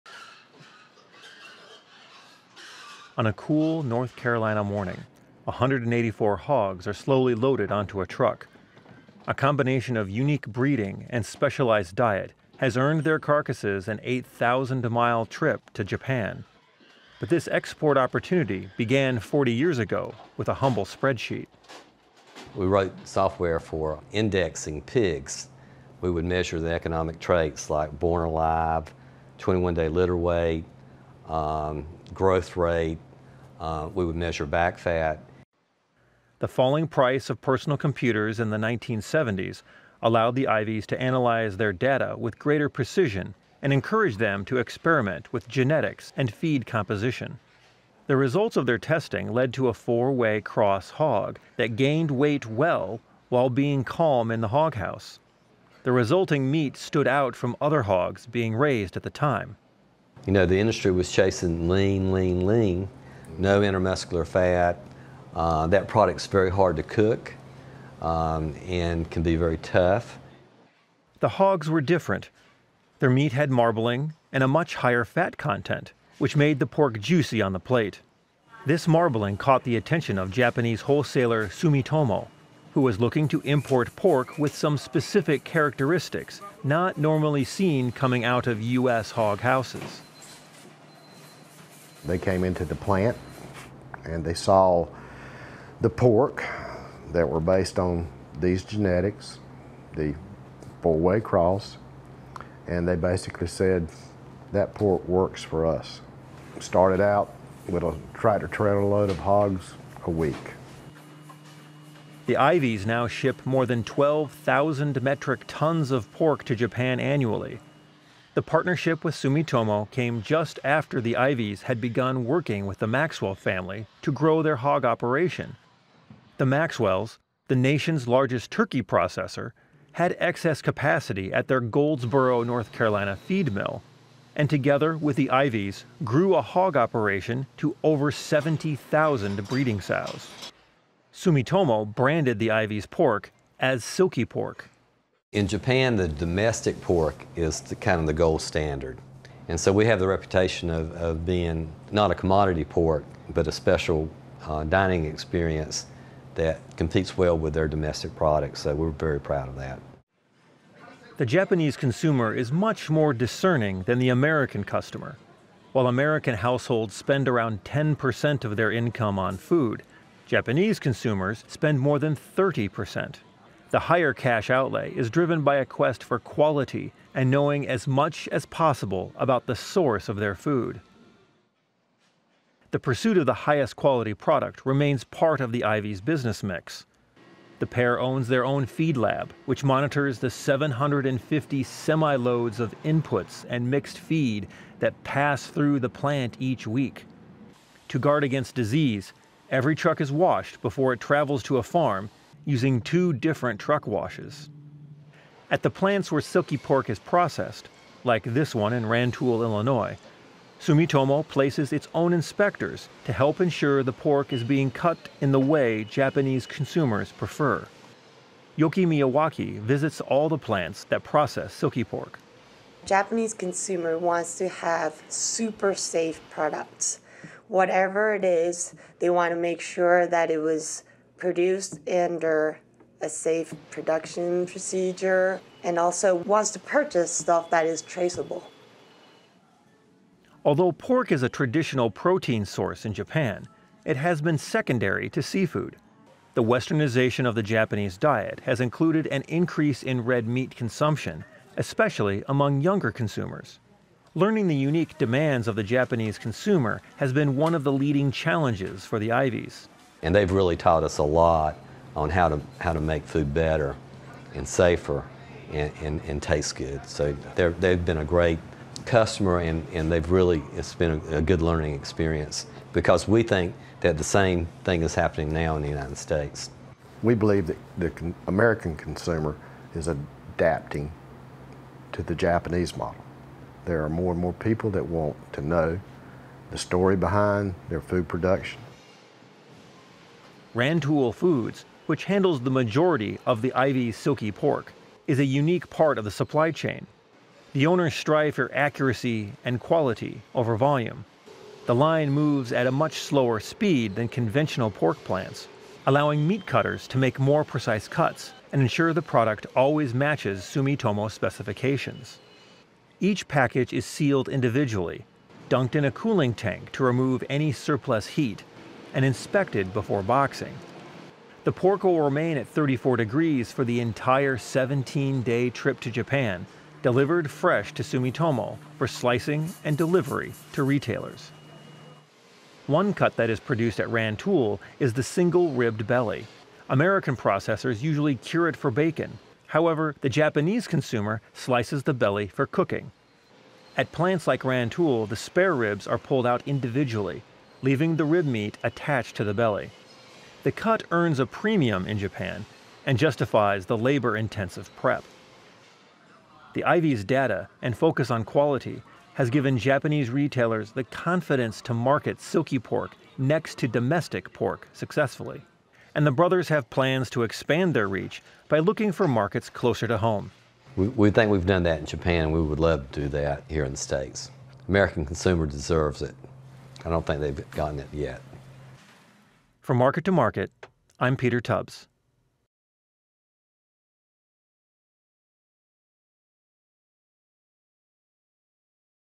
Specialty Pork Finds Japanese Audience
Making a business flourish is sometimes a chance meeting or pursuing a path others have long-since abandoned. For two North Carolina brothers it was a combination of both that started a profitable venture based on a single idea.